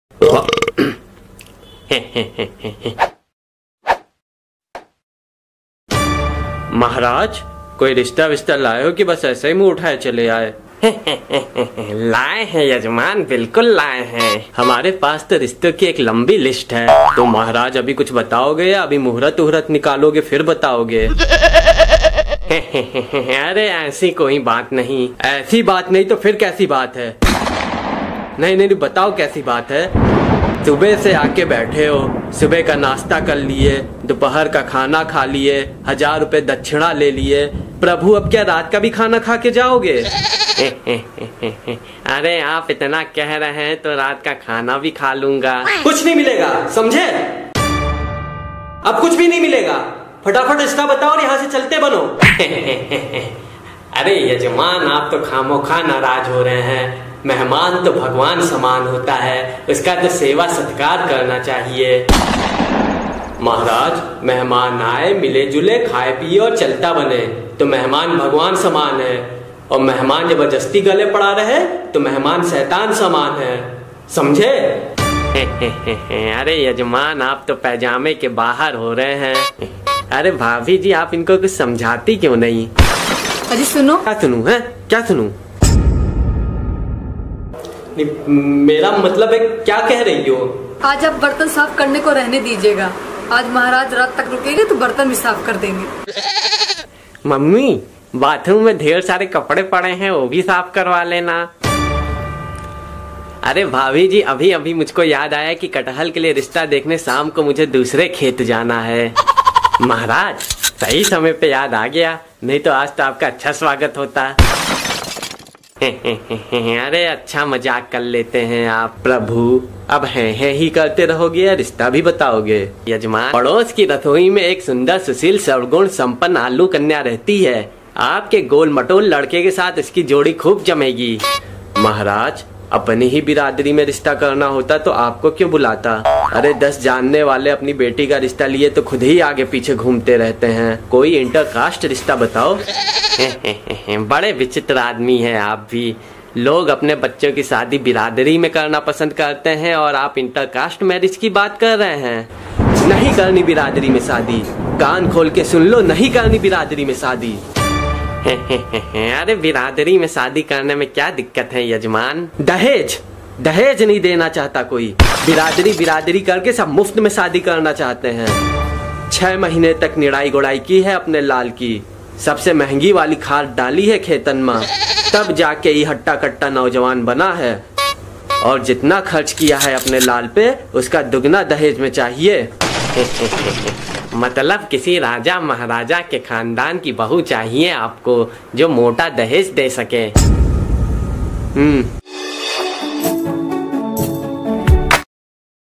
आलु का रिस्ता || Aloo kaa rista || Funny comedy 🤣😂
The aloo family called up purohit to get his sun married                                                                         your queries :-                                                              aloo ka rista comedy                                                 aloo ki beti ka rista                                               comedy video                                                         aalu cartoon                                                              funny video
hindi comedy
hindi cartoon
hindi video